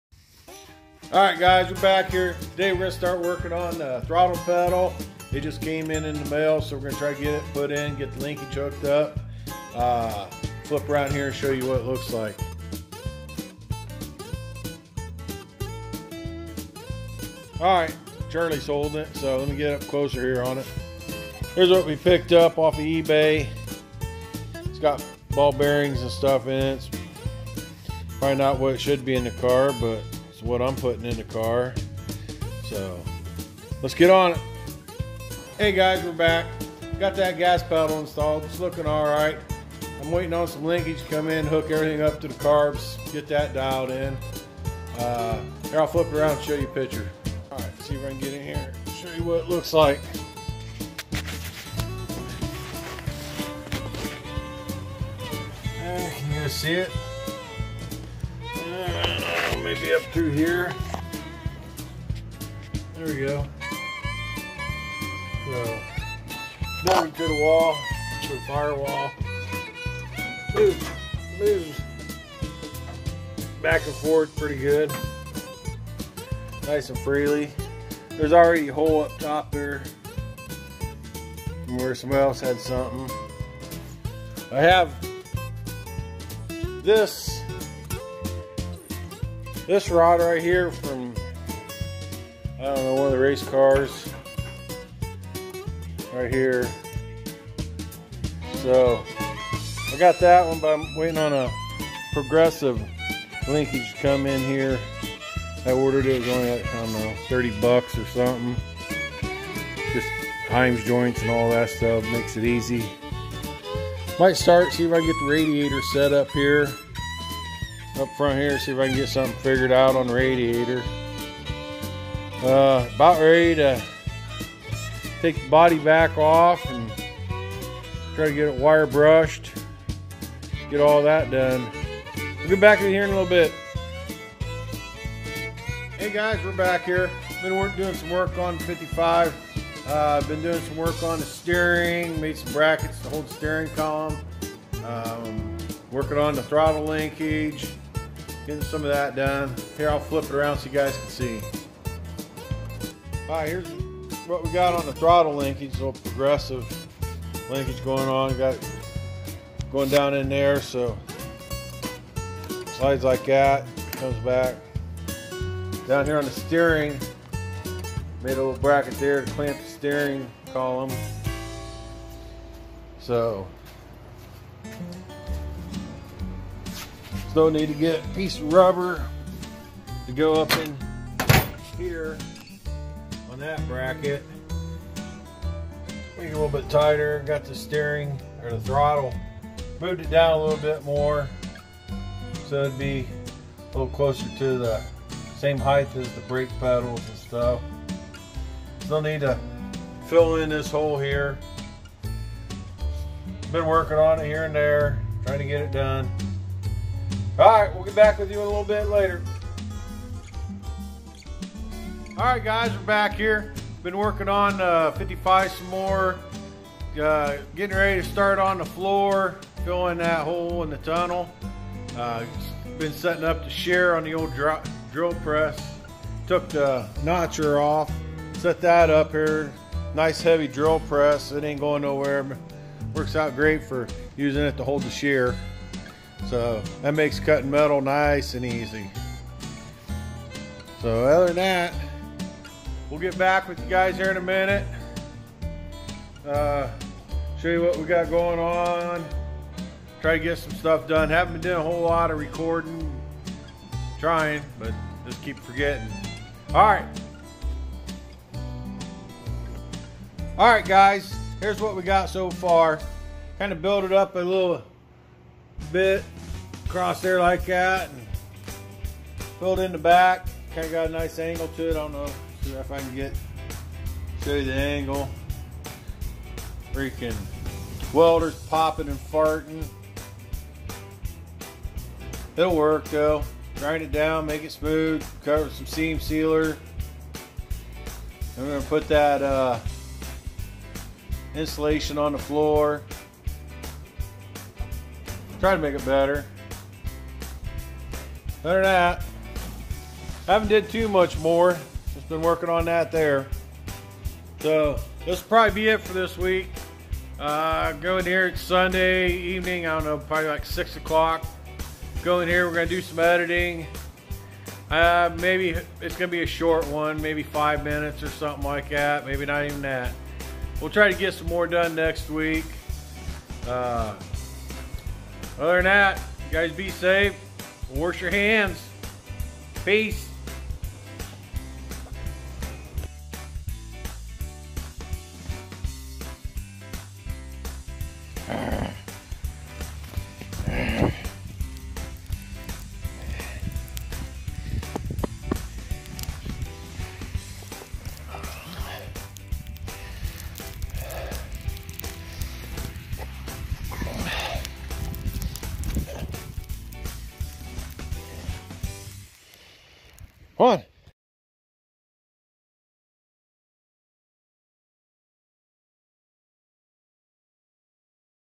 2020 Power TOUR BUILDS GASSER EPS 9 11 WEEKS TO GO
Working on throttle leakage  steering  an floor